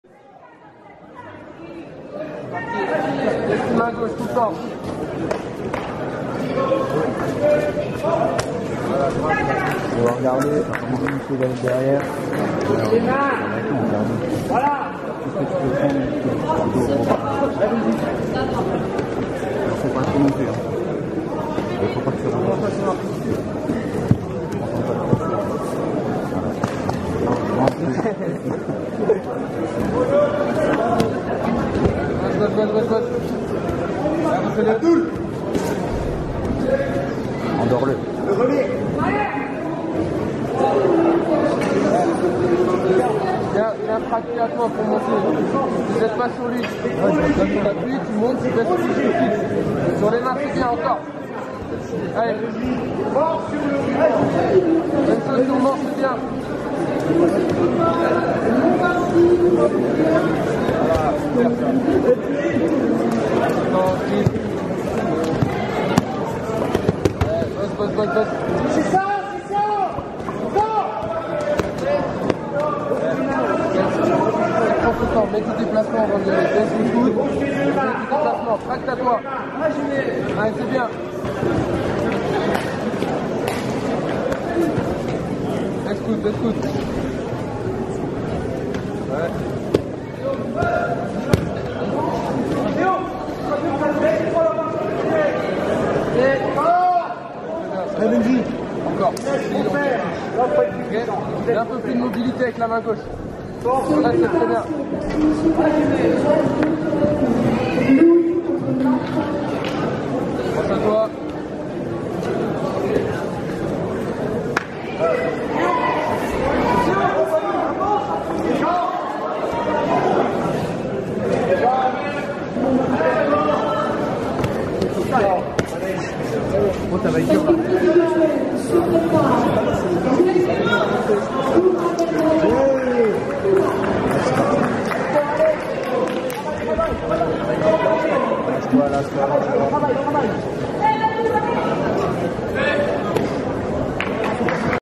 Benjamin Breitbeil - Championnat Juniors Grand Est - 11 février 2024 - Saverne - 1er combat
Benjamin Breitbeil
Championnat Juniors Grand Est
11 février 2024
Saverne
JUDO BSM
BSM